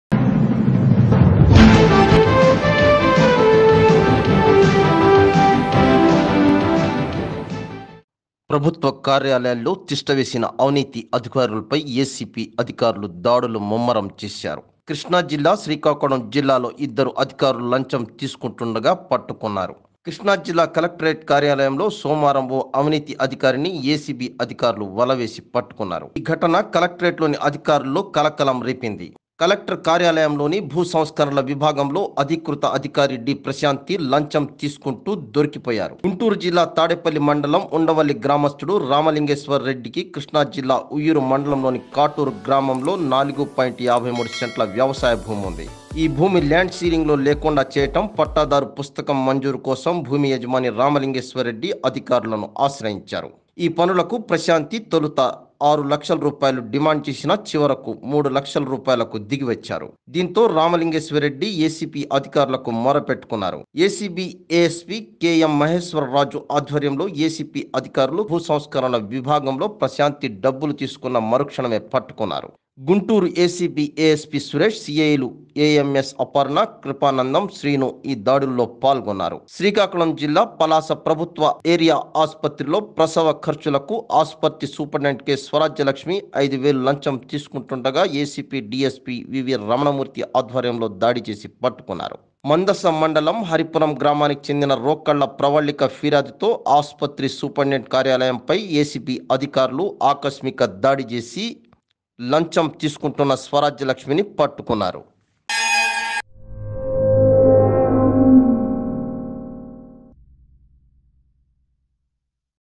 APTODAYDIGITALTV అక్రమార్కులపై ..ACB కొరడా..!ఆ ఇద్దరు ఎలా చిక్కారంటే
అక్రమార్కులపై ..ACB కొరడా..!ఆ ఇద్దరు ఎలా చిక్కారంటే